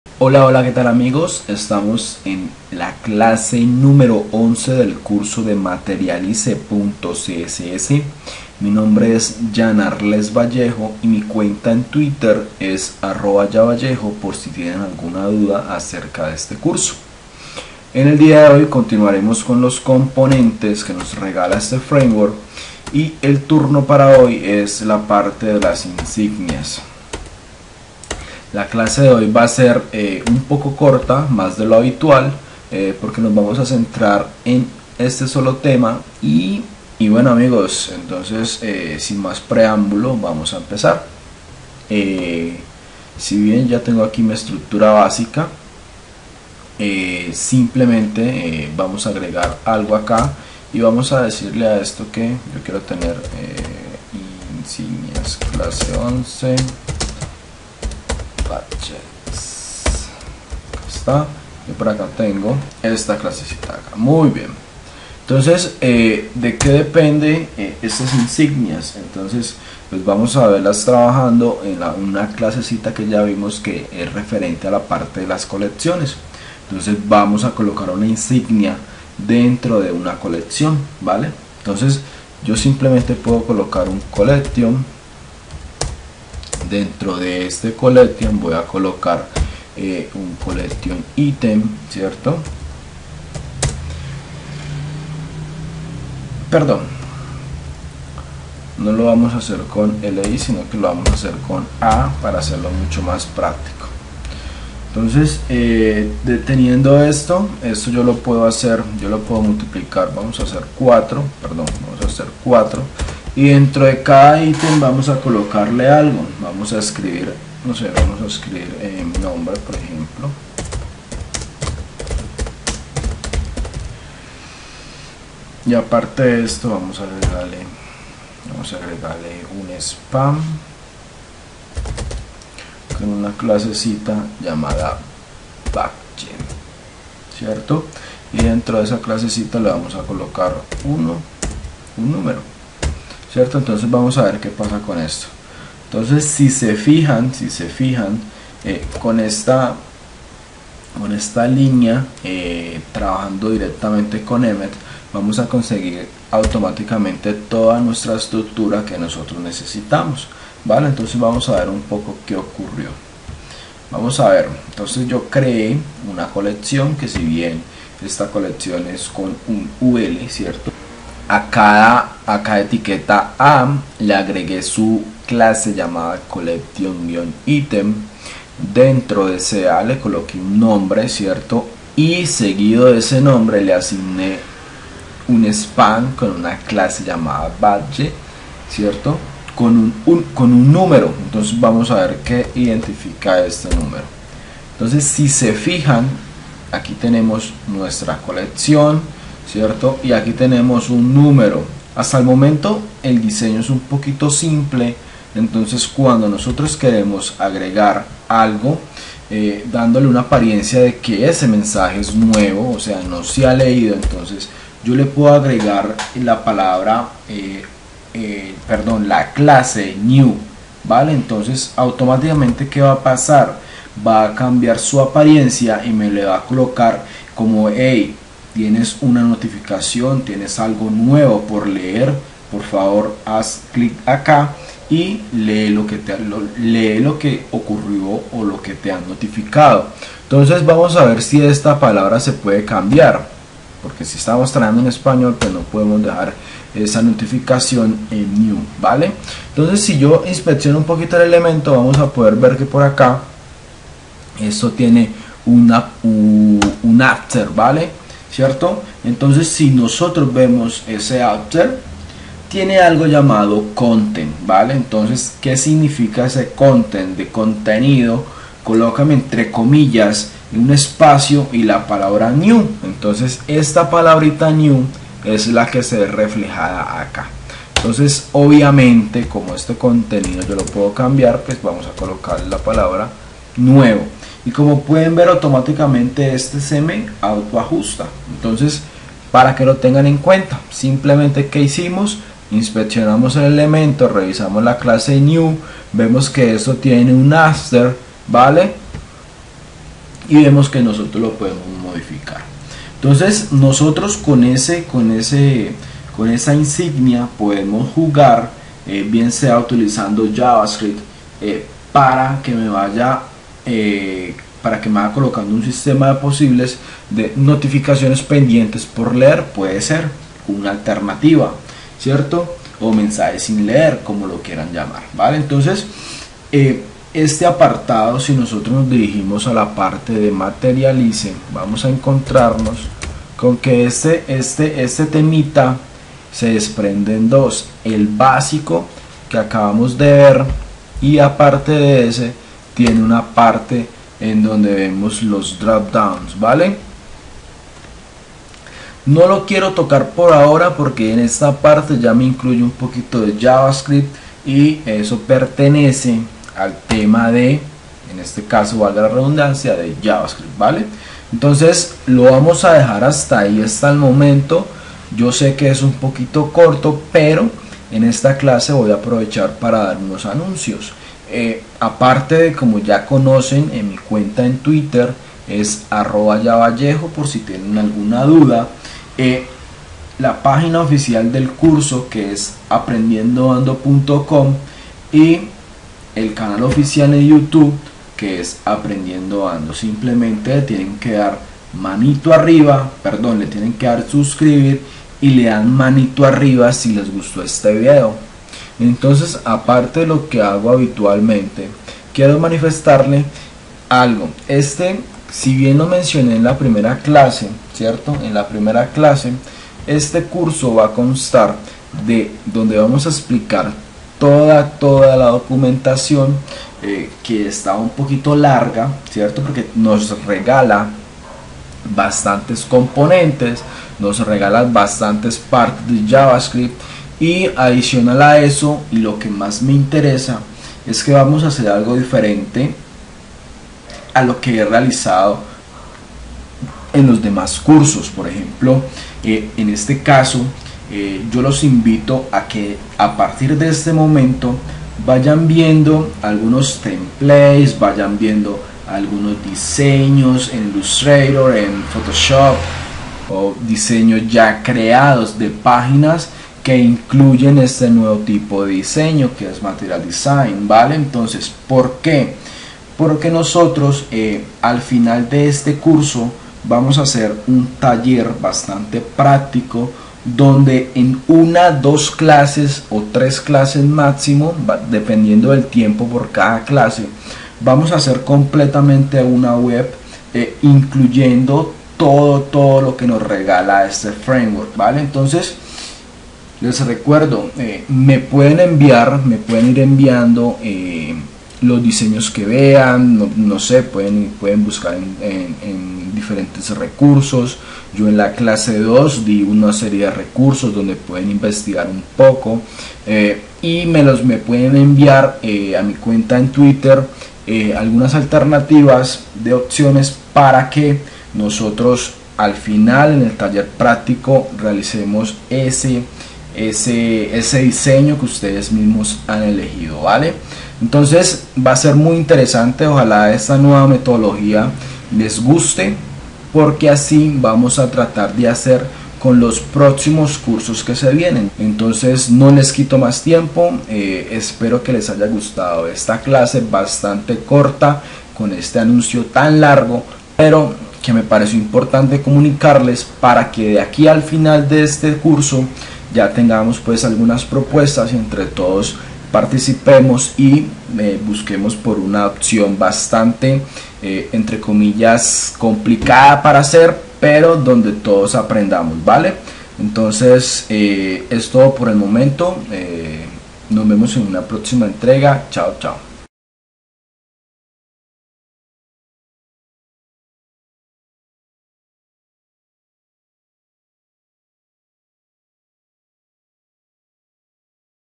MaterializeCSS - Clase 11 - Componentes : Badges
Hola que tal amigos en esta clase número 11 del curso de MaterializeCss continuaremos con los componentes y es el turno para los Badges o  insignias, el cual utilizaremos en su forma básica, También daremos un breve anuncio sobre el final de este curso, para que participen y nos ayuden a construir un taller final donde aplicaremos todo lo aprendido de este Framework CSS

Donaciones

Yan Arlex Vallejo.